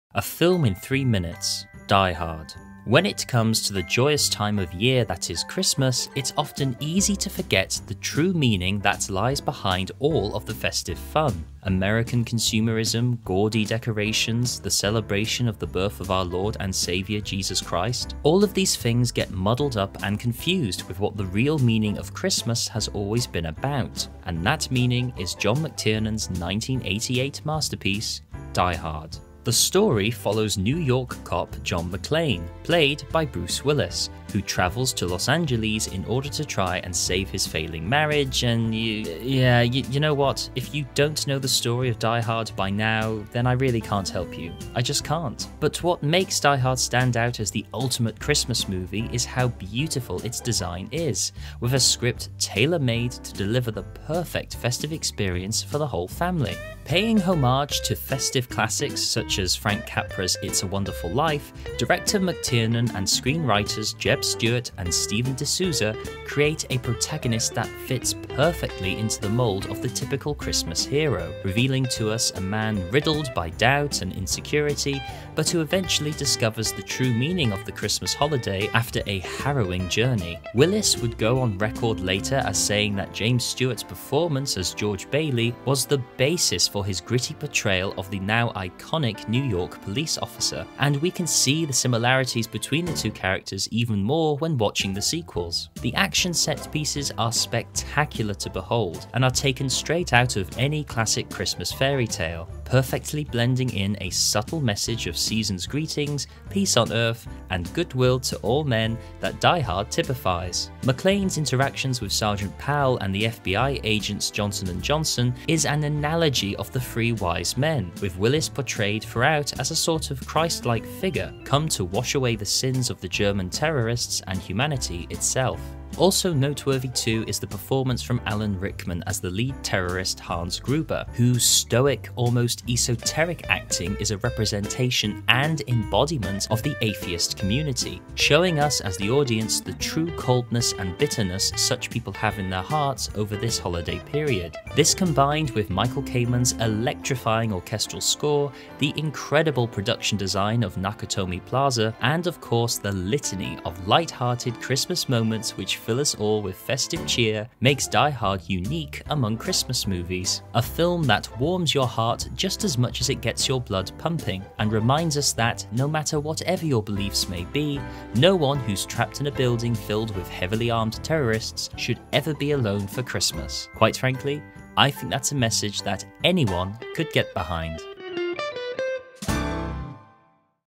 Die Hard - A Film in Three Minutes
Get into the festive spirit with humanity's greatest ever Christmas movie, Frank Capra eat your heart out! NOTE: Some of the things mentioned in this video MIGHT not be true!